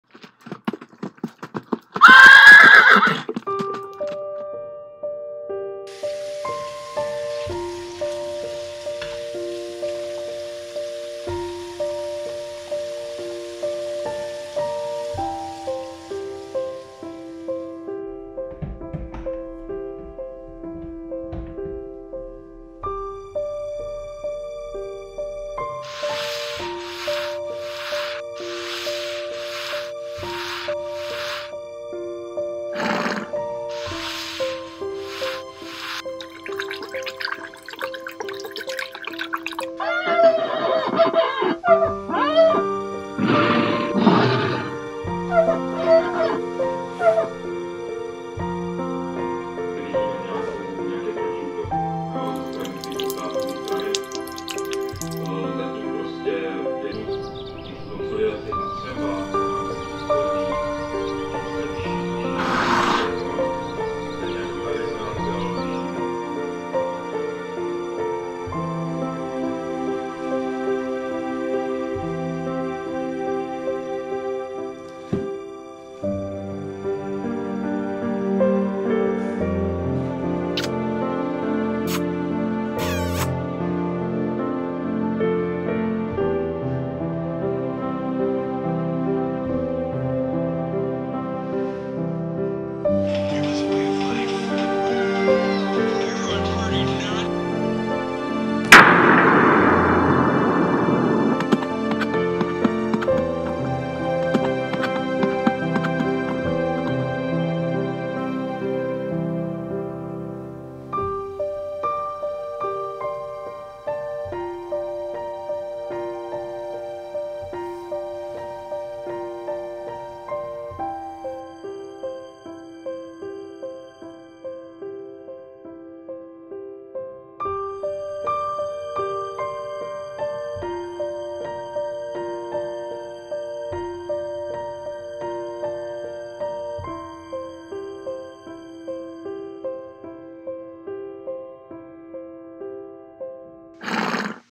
HORSE in Covid Isolation - Day 14
Silný příběh o jednom koni a jeho čtrnáctidenní covid izolaci  :)

Věnováno všem, kdo i bez příznaků poctivě dodrželi 14 denní izolaci a obětovali část svého psychického zdraví ve prospěch fyzického zdraví ostatních :)

Zvláštní věnování pak Michalu Maierovi! :D